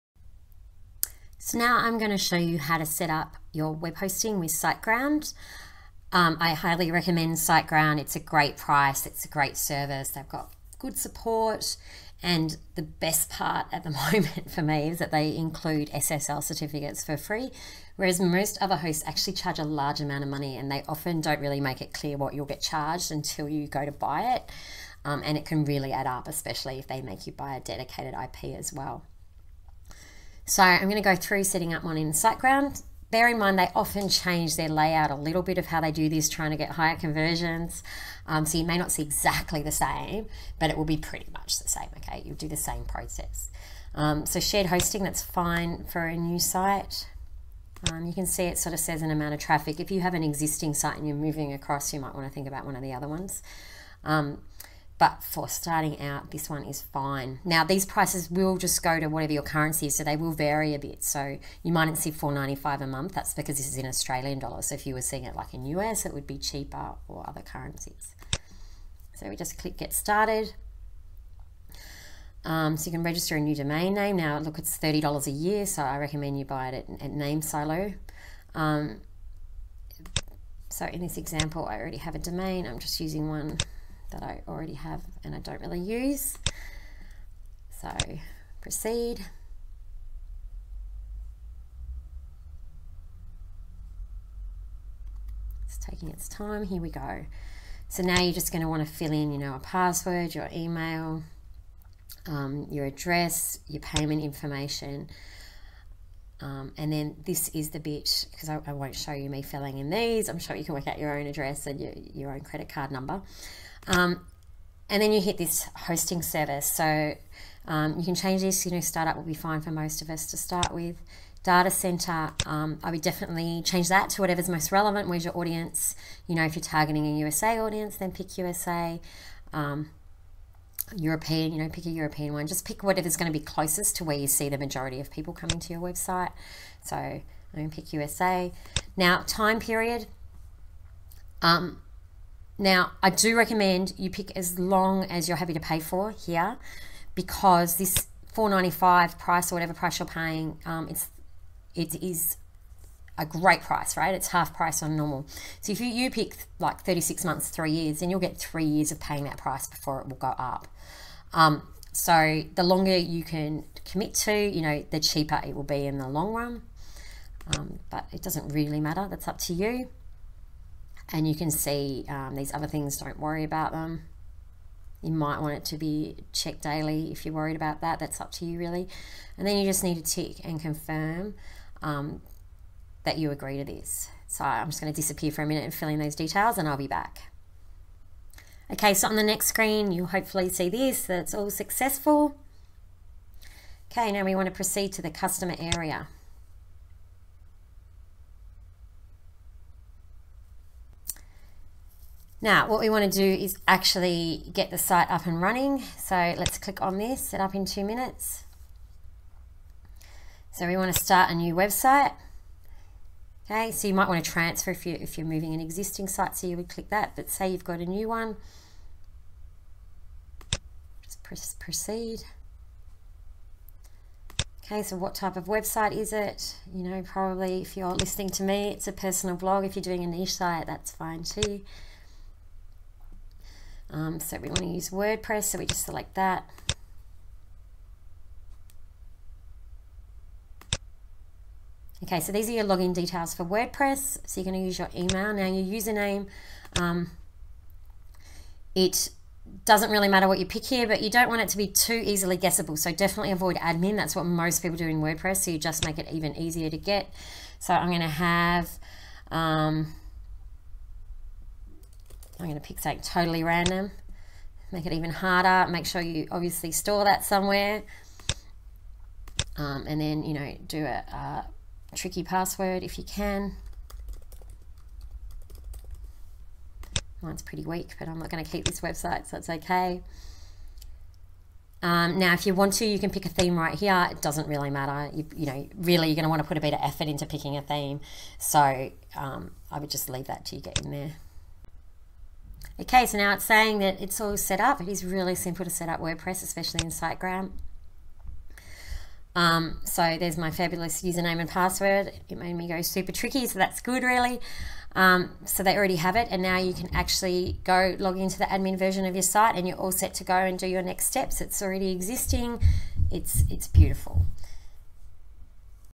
How To Set Up Hosting With SiteGround: How to Start a Blog
A step by step guide on how to set up hosting with SiteGround.  Followed my tutorial where I show you a step by step guide to setting up hosting on SiteGround.  This includes how to sign up with SiteGround as your host and this includes how to sign up with SiteGround as your host.

This video is part of my guide on how to start a blog or niche site.  You can read more including full instructions here: